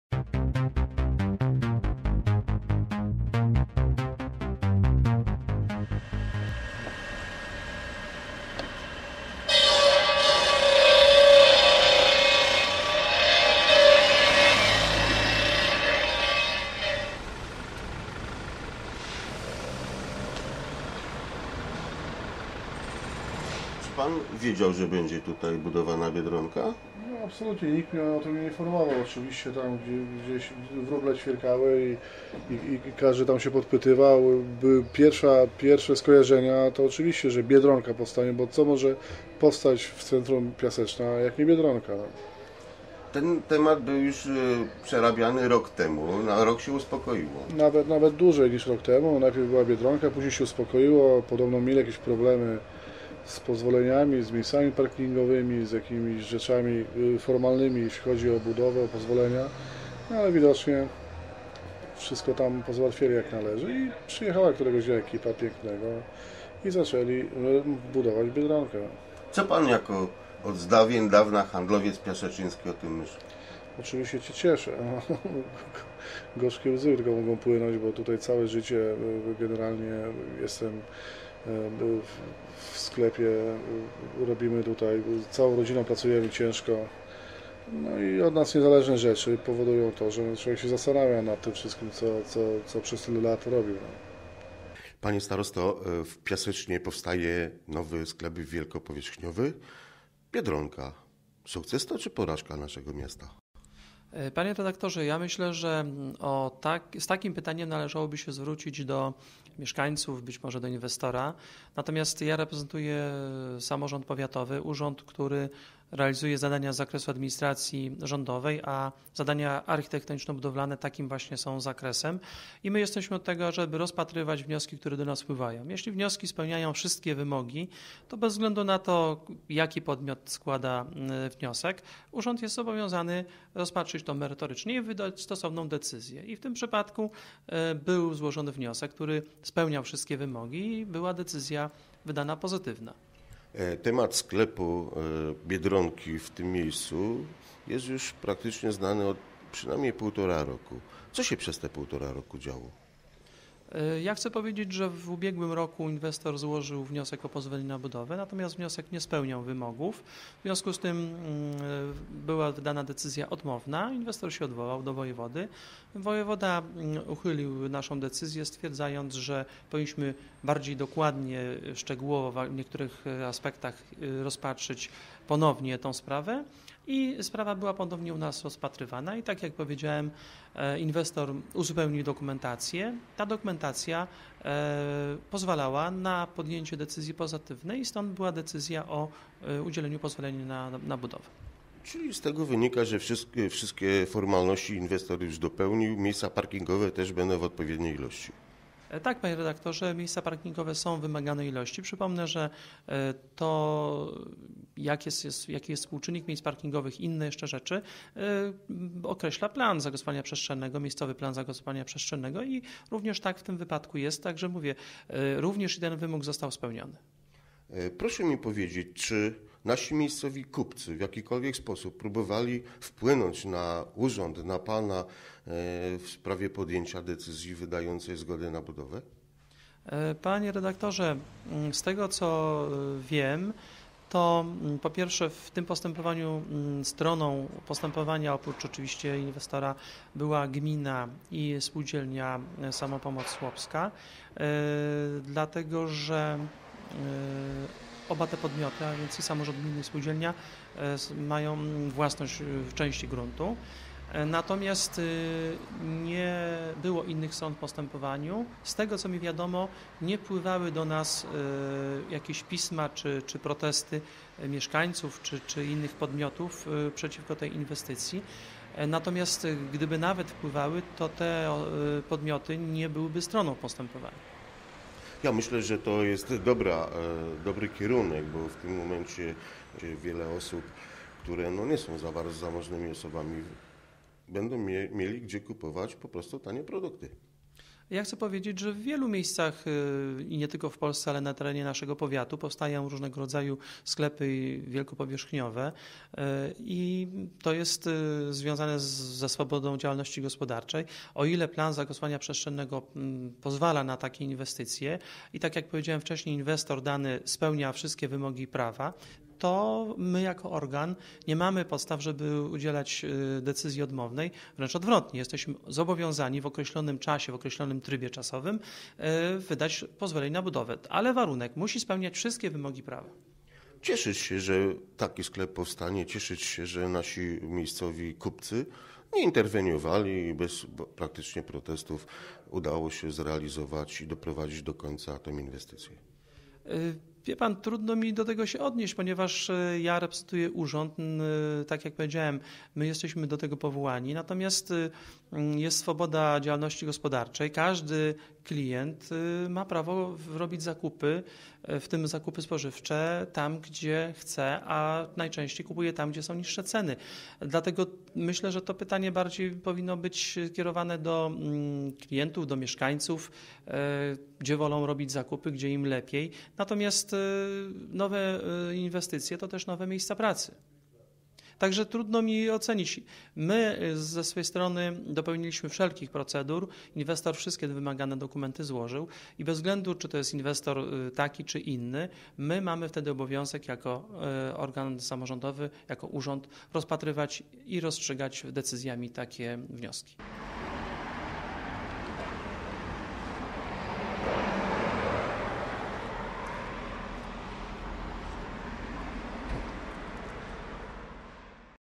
Budowa Biedronki w Piasecznie
Półtora roku potrzebował inwestor, aby zdobyć niezbędne dokumenty pozwalające postawienie Biedronki w Centrum Piaseczna. Otwarcie we wrześniu. Stanie na terenie dzierżawionym od Gminy oraz Samopomocy Chłopskiej.